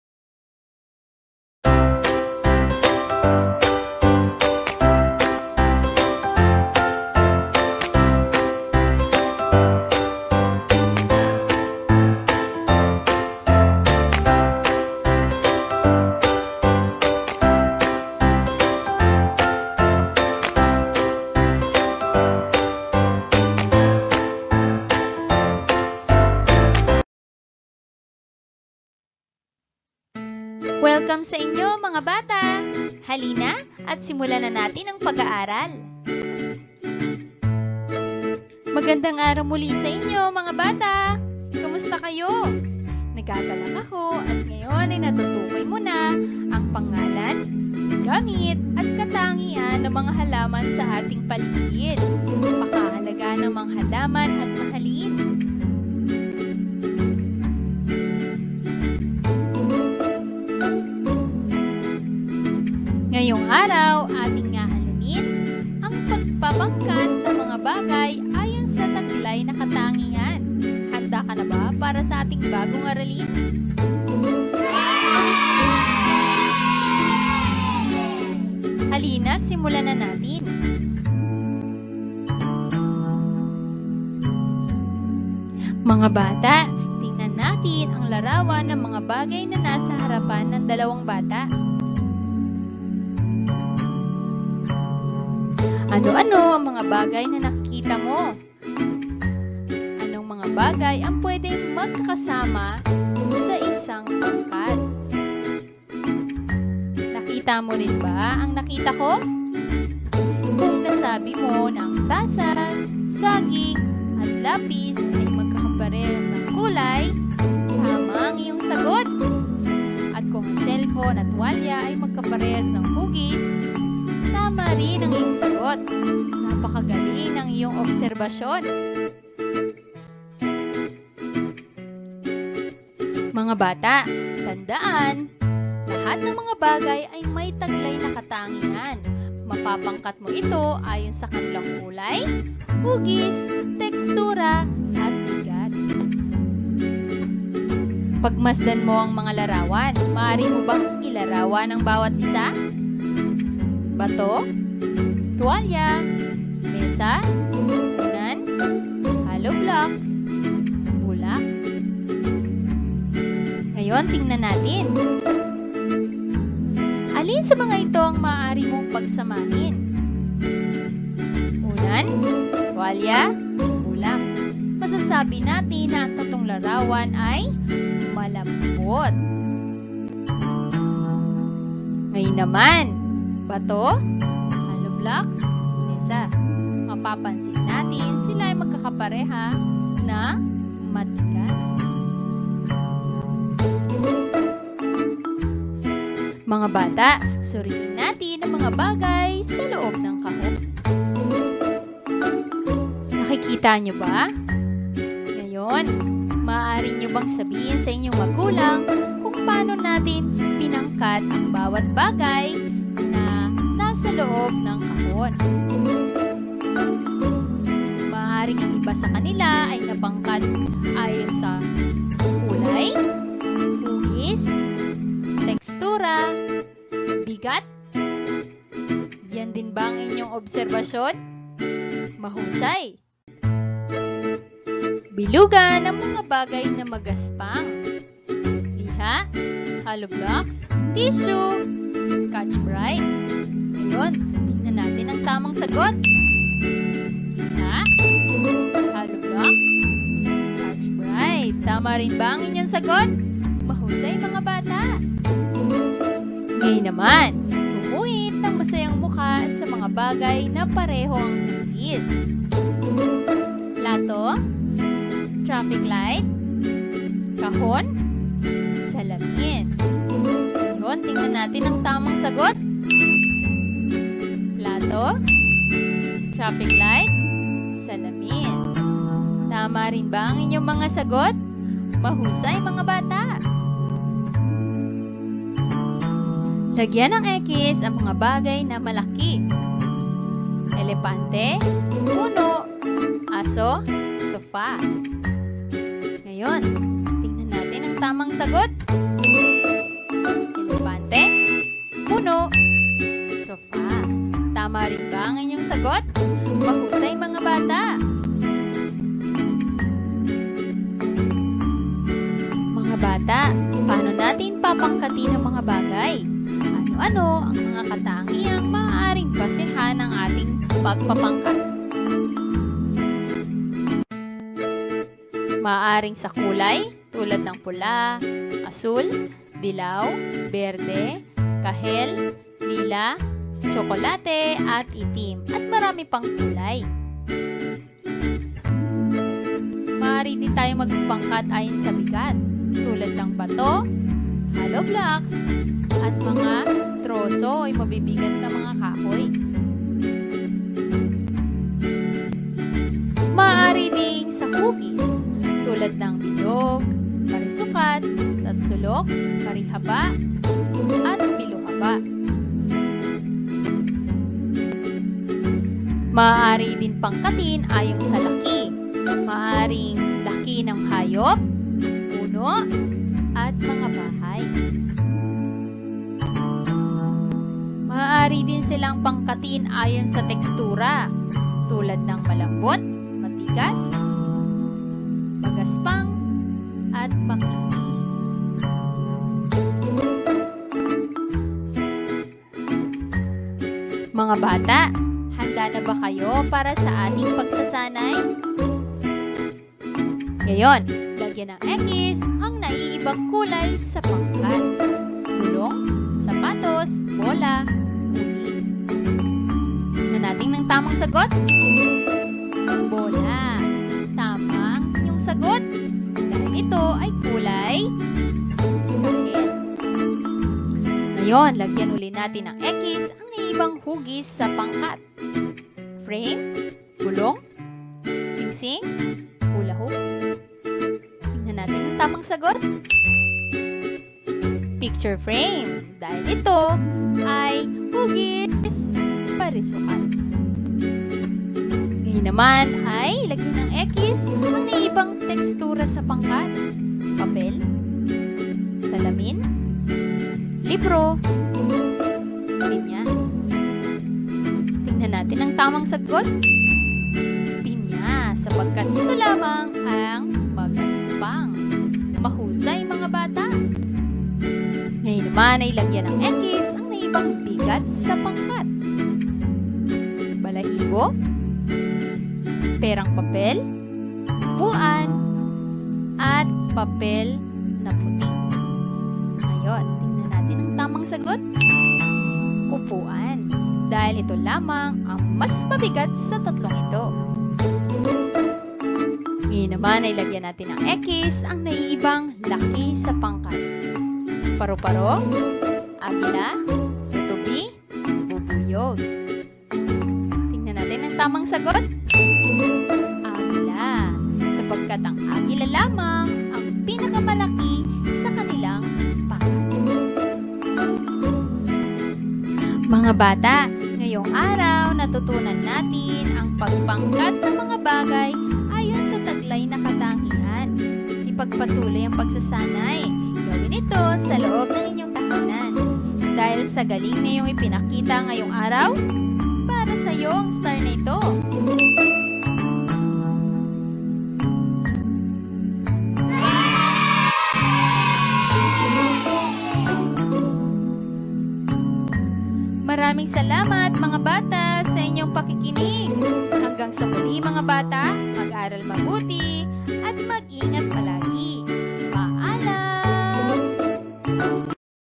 Kindergarten Q4 Week 3 Modyul 1
Pagpapangkat ng mga bagay ayon sa taglay na katangian